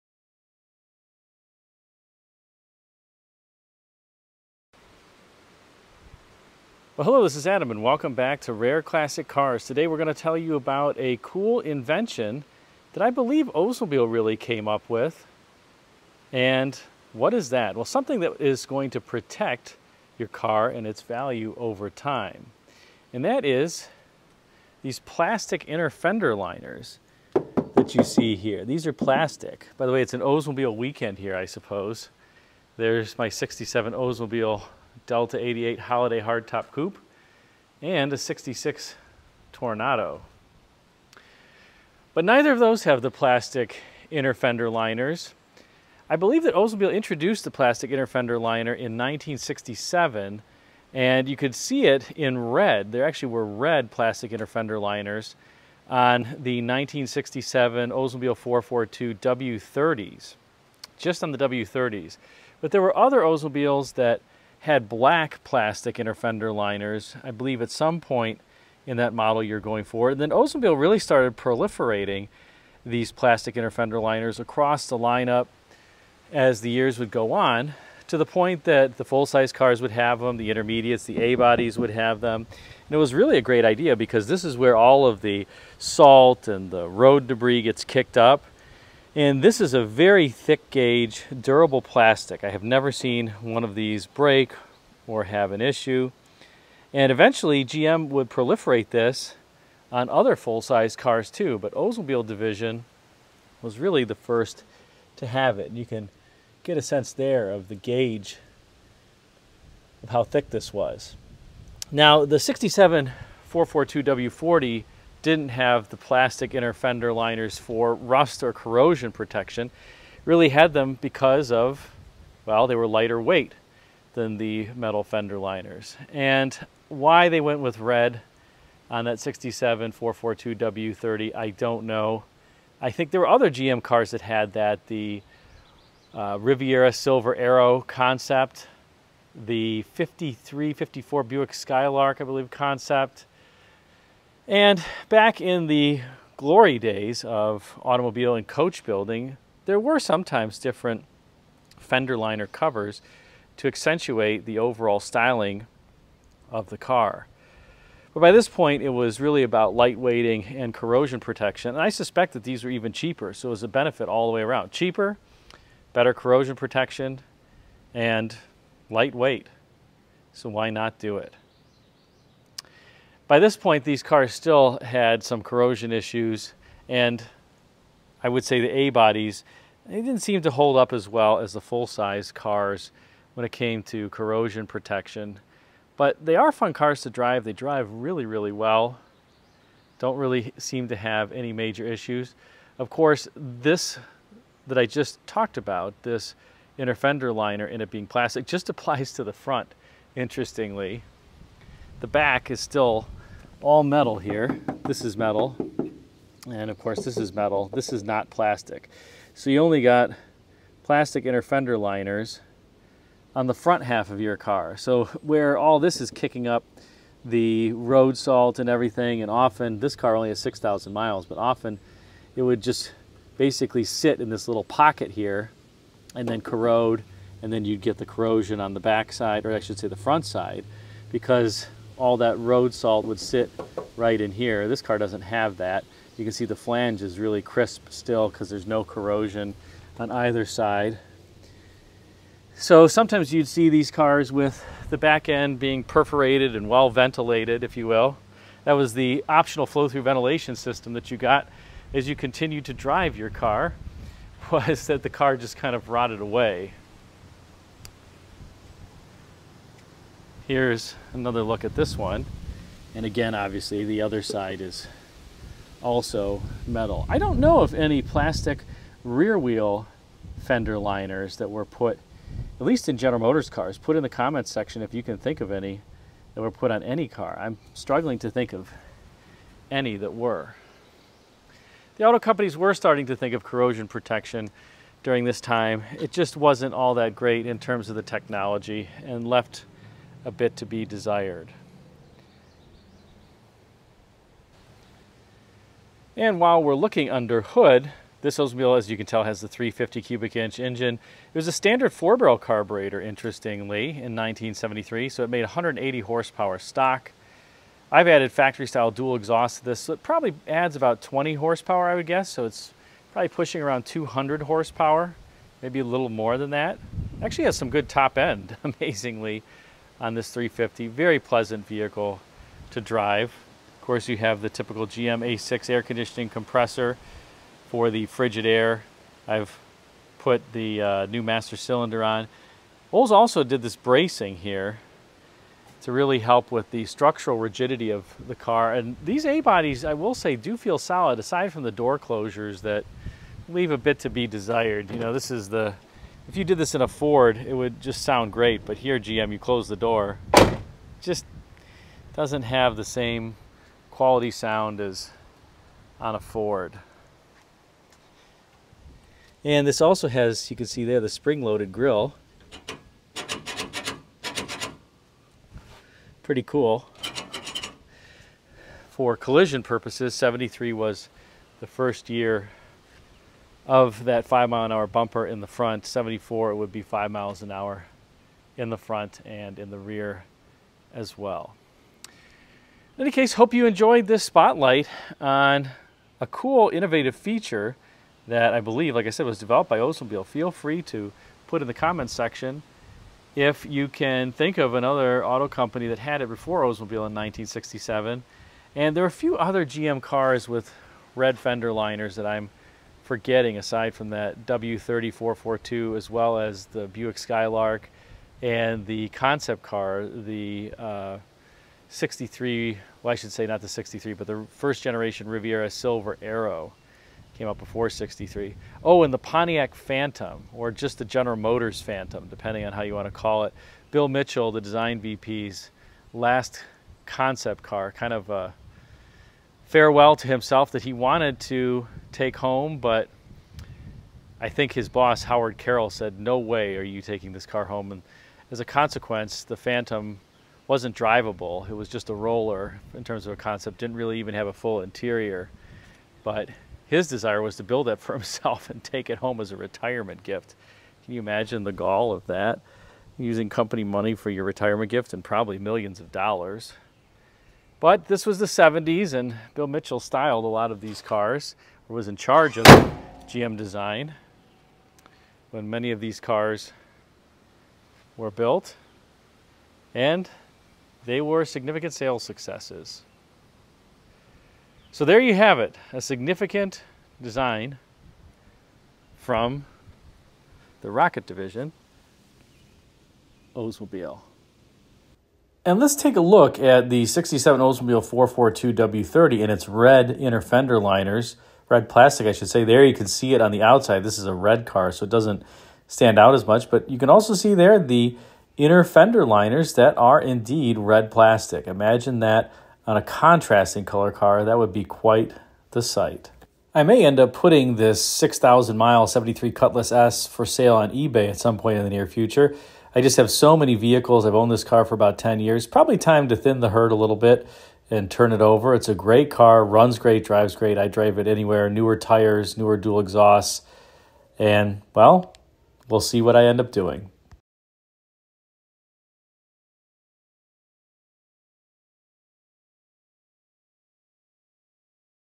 This Oldsmobile Invention Protected Your Car and Preserved Its Value!
Learn more about this Oldsmobile invention that began in 1967: plastic inner fender liners.